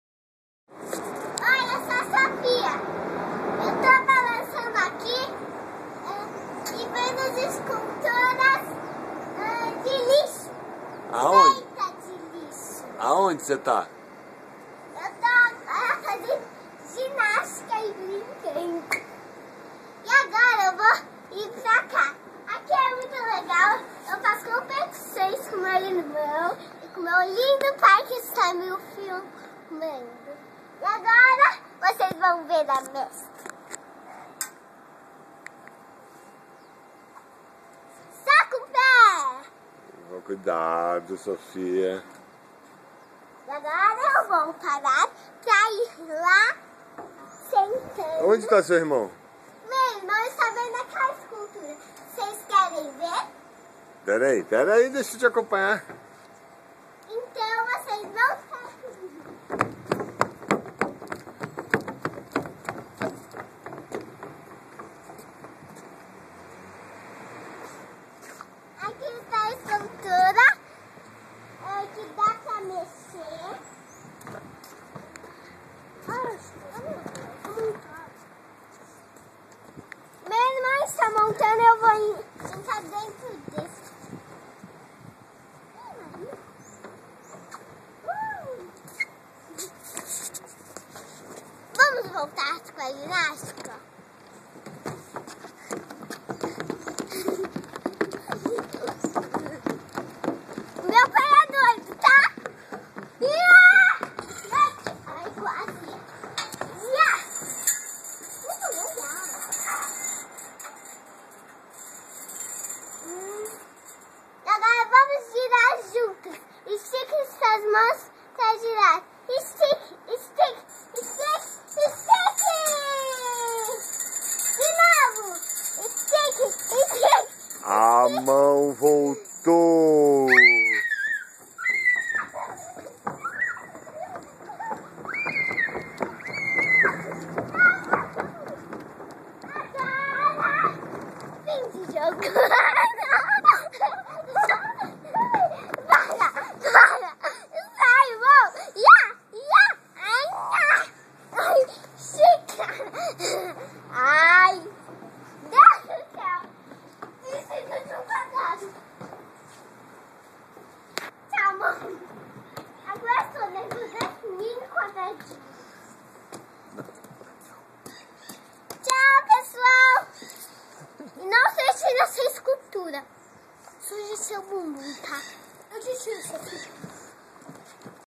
Expo Lixo Reciclável
Sofia apresenta nosso passeio a uma exposição de lixo reciclável em praça em SP.